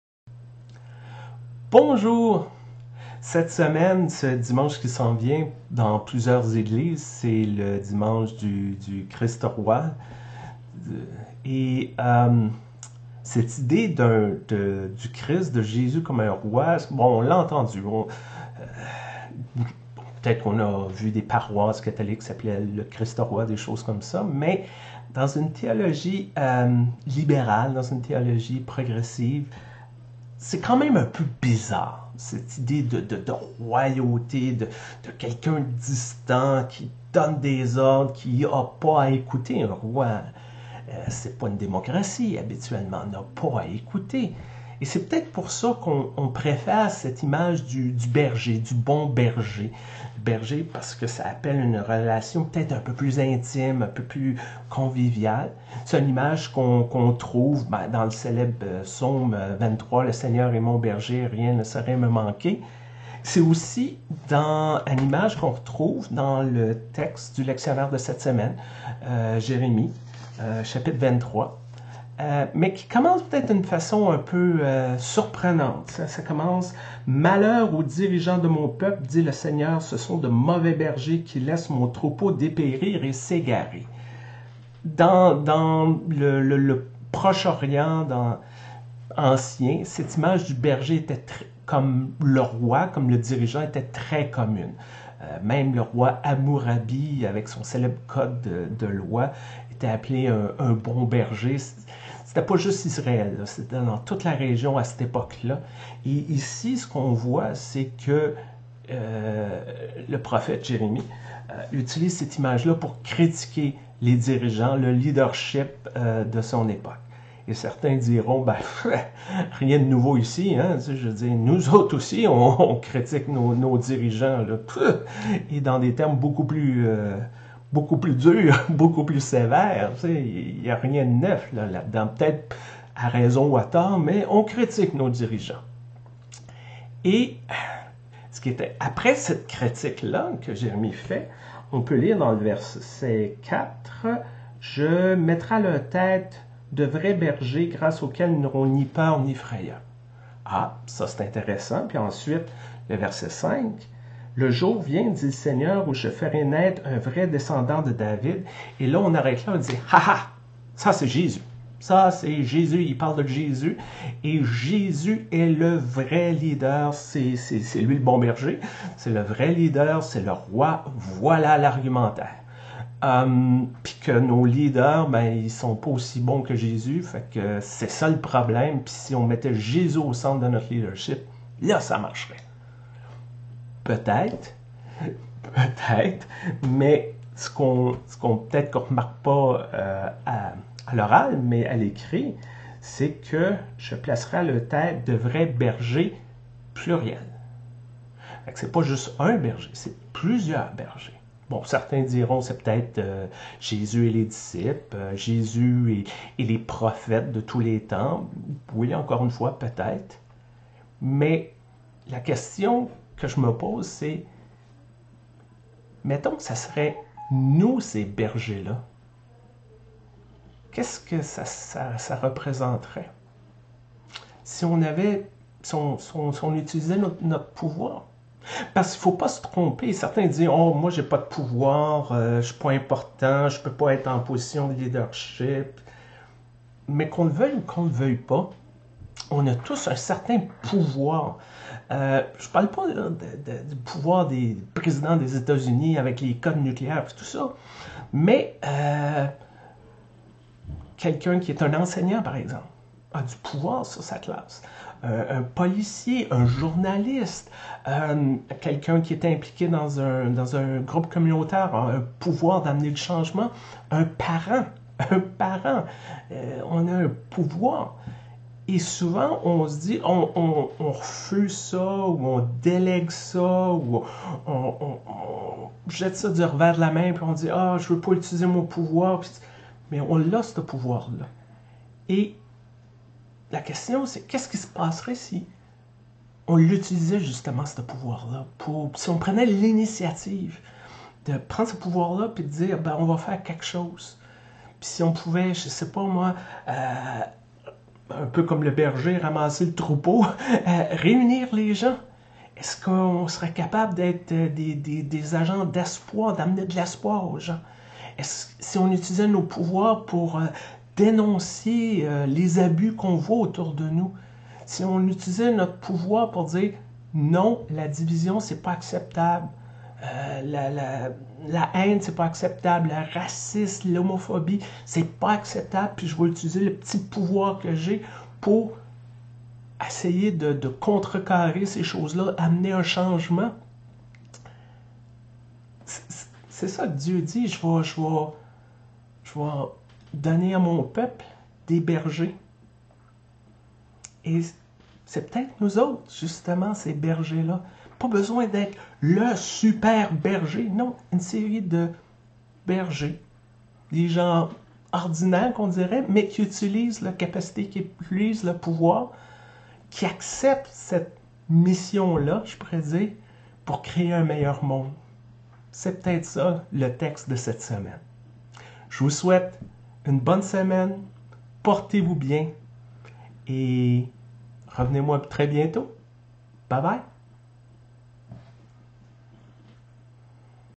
Jérémie 23: 1-6
«Quel malheur! dit le Seigneur.  Les dirigeants de mon peuple sont de mauvais bergers, qui laissent mon troupeau dépérir et s'égarer.»  Voici donc ce que déclare le Seigneur, le Dieu d'Israël, au sujet de ces bergers: «Vous avez laissé mon troupeau s'égarer et se disperser.  Vous ne vous êtes pas occupés de lui.  Eh bien, moi, je vais m'occuper de vous et de vos agissements, dit le Seigneur!  «Je vais rassembler moi-même les survivants de mon troupeau, dans tous les pays où je les ai dispersés.  Je les ramènerai à leur pâturage, où ils pourront prospérer et se multiplier.  Je mettrai à leur tête de vrais bergers, grâce auxquels ils n'auront plus ni peur ni frayeur.  Aucun d'eux ne manquera plus à l'appel, dit le Seigneur.»  «Le jour vient, dit le Seigneur, où je ferai naître un vrai descendant de David.  Il sera un roi compétent, il agira dans le pays selon le droit et l'ordre que je veux.  Quand il régnera, Juda sera libéré, Israël vivra tranquille.  Voici le nom qu'on lui donnera: “Le Seigneur est notre salut” .